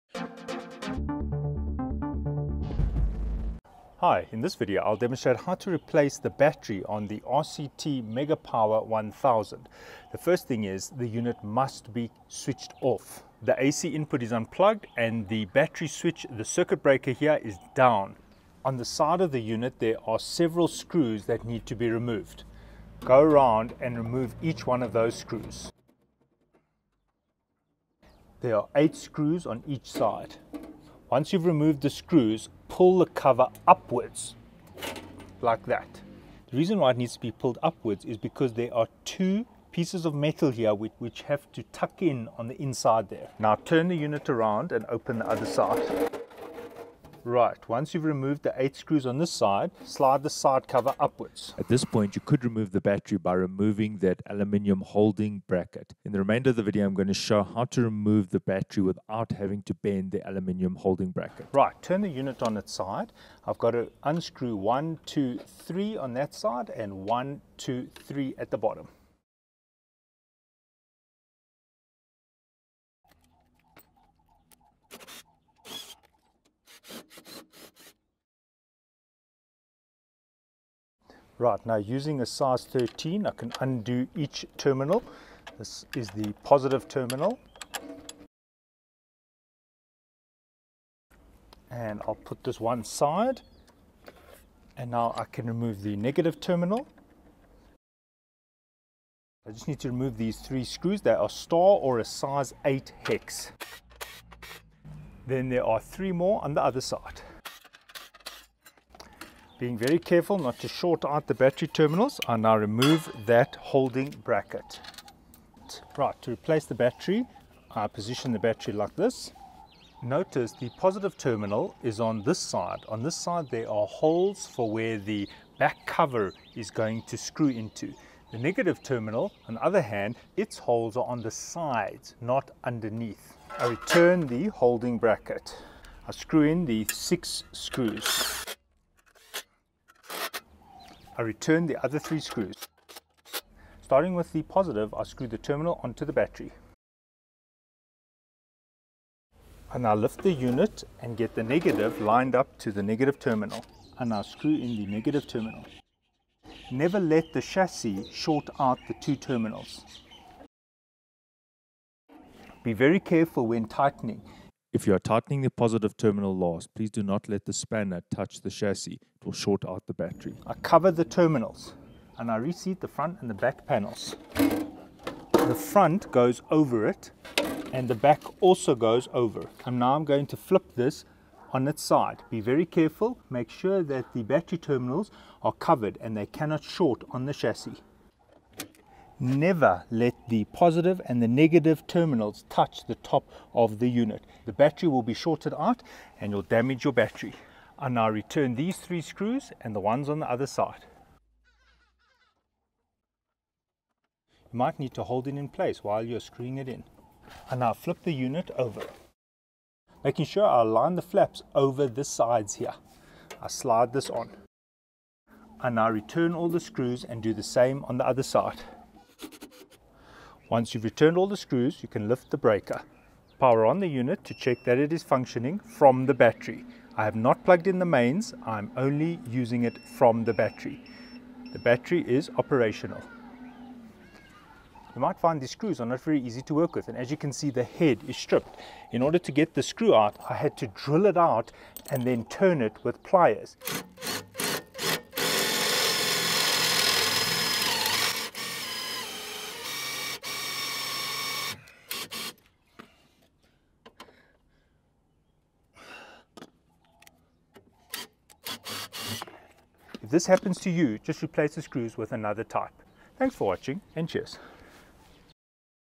How to change the Gel lead acid battery on the RCT MegaPower 1KVA/1000W Inverter Trolley
RCT MegaPower 1KVA/1000W Inverter Trolley With 1 x 100AH Battery
Kool Energy 1KW Plug And Play Inverter System All In One